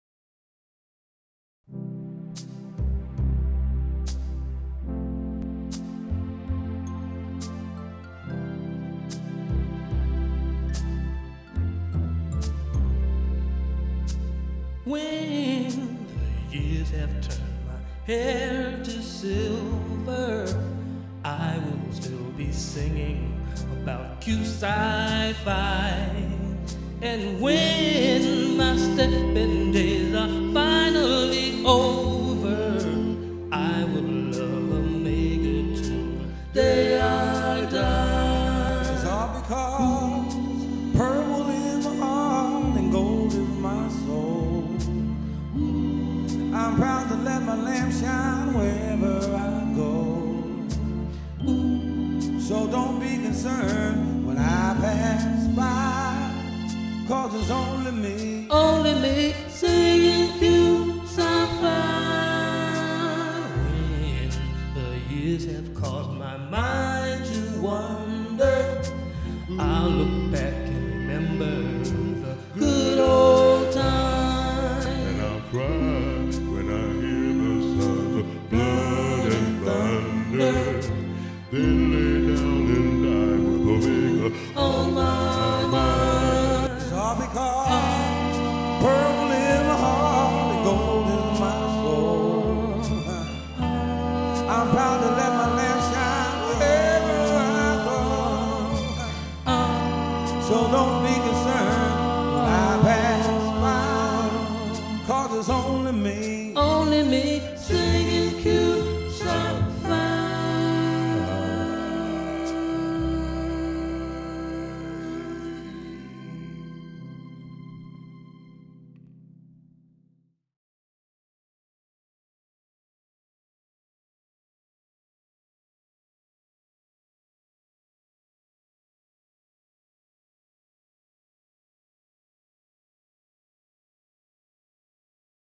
"When the Years" Mu Delta.mov
Early History of Mu Delta Chapter of the Omega Psi Phi Fraternity, Inc., founded at the University of Iowa.